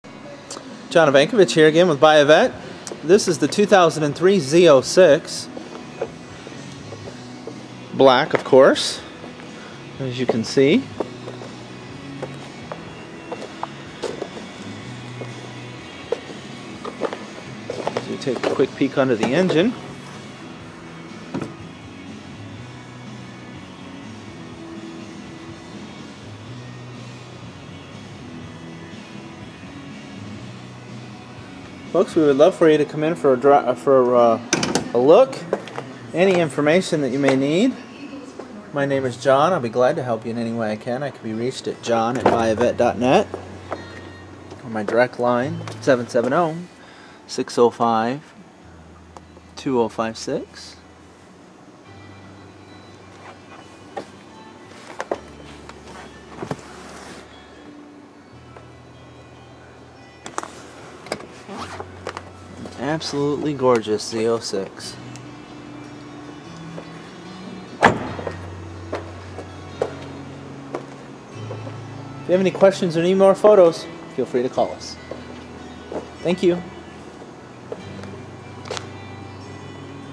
2003 Corvette Z06
2003 Corvette for sale. Black exterior, Black interior. 405hp LS6 engine, 6 speed manual transmission, posi rear-end.

9 ACTUAL MILES!!